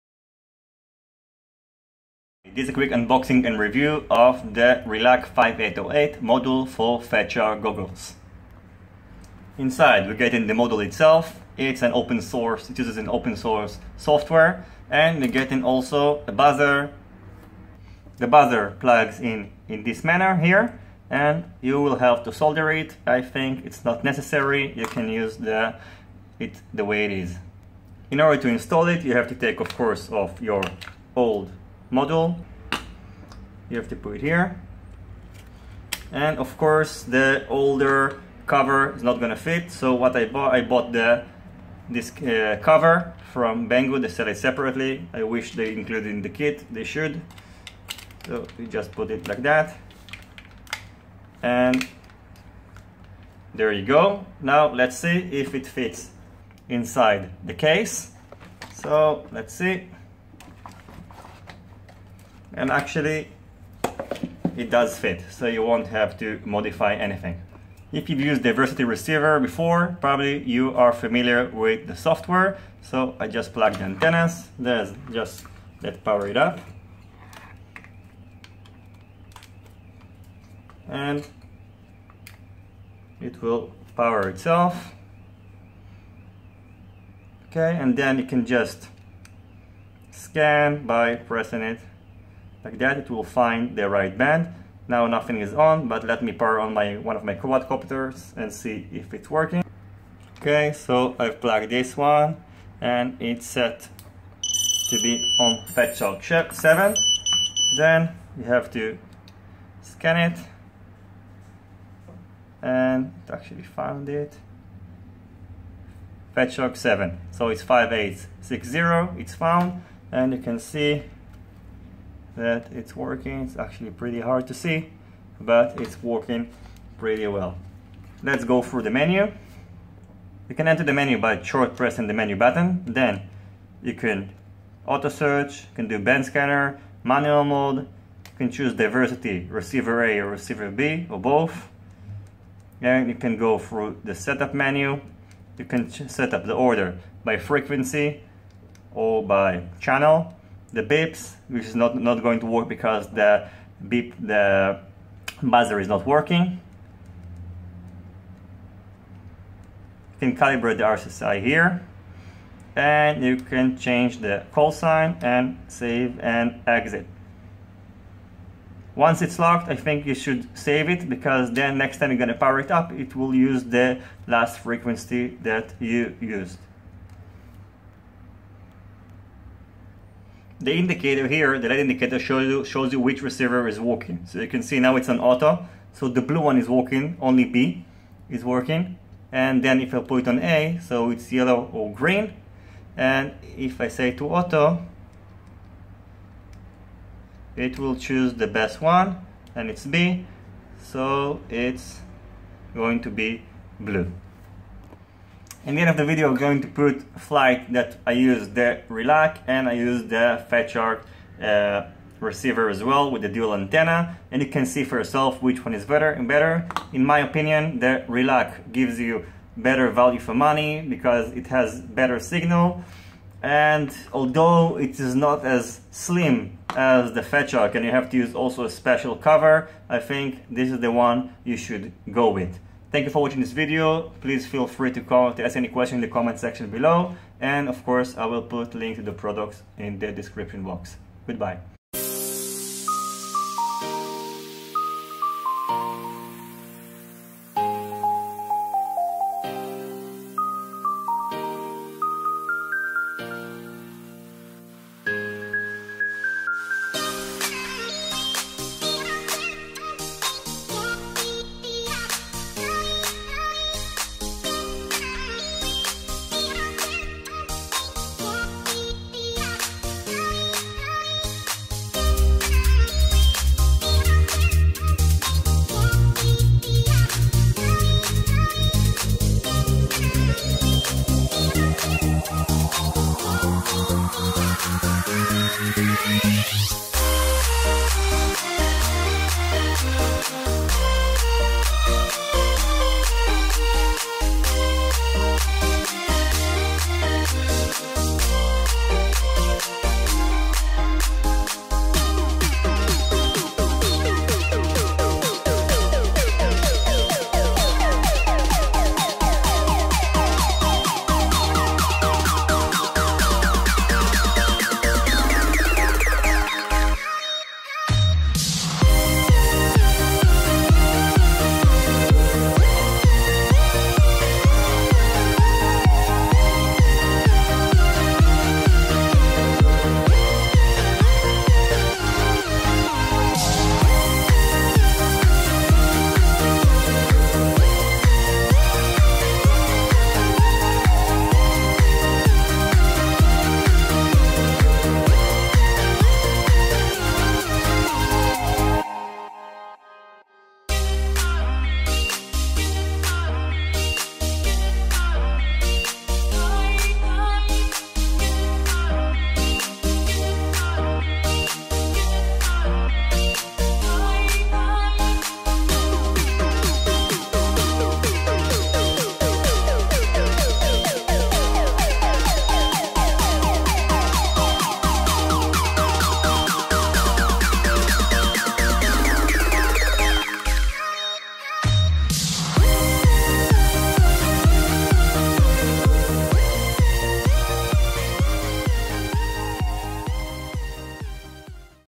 Realacc RX5808 Pro Diversity Open Source 5.8G 40CH Integrated OLED unboxing and review
Description:
Brand Name: Realacc
Item Name: RX5808 Receiver
Frequency: 5.8G
Channel: 40CH
All in one integrated receiver
Special for Fatshark Goggles
With OLED display
Auto scan
Channel switch in 25ms

Package Included:(antennas are not included)
1 x Receiver
1 x Cover